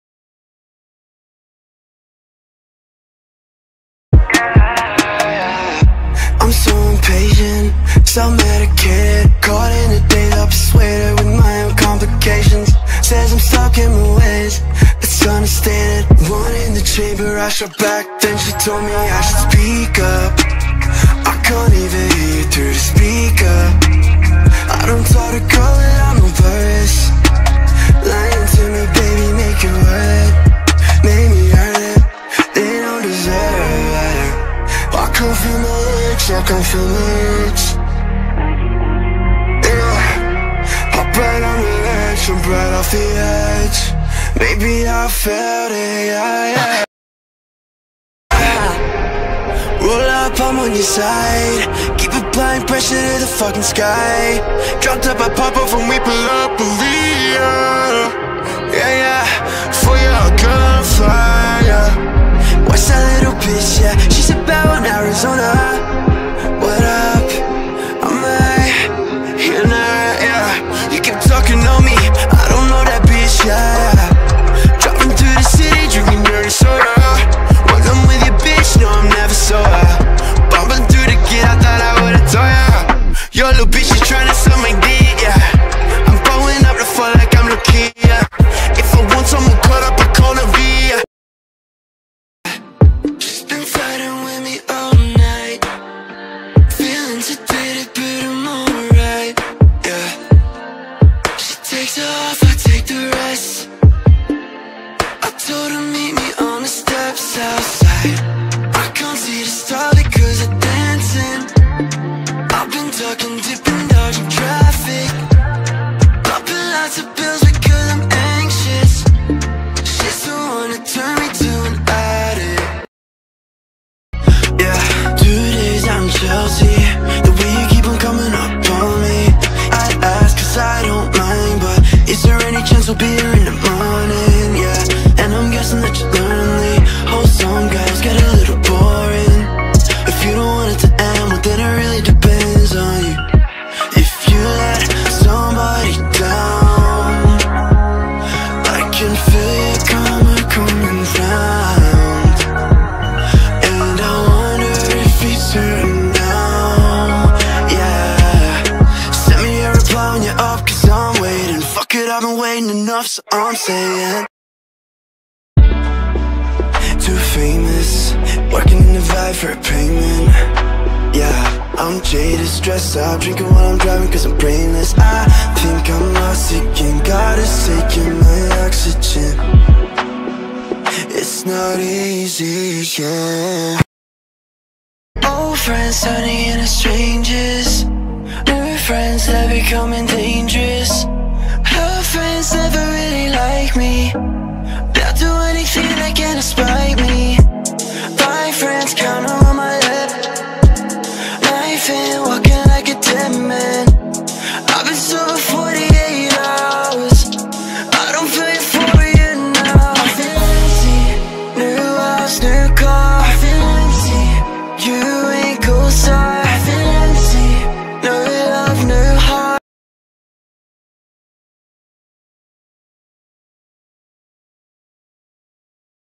every christian anthony solo ever (so far) BEAUTY IN DEATH edition
surprise 🤪

I needed a video bc I've been lazy and haven't edited anything I've filmed and yeah so here's this also because it was requested a few times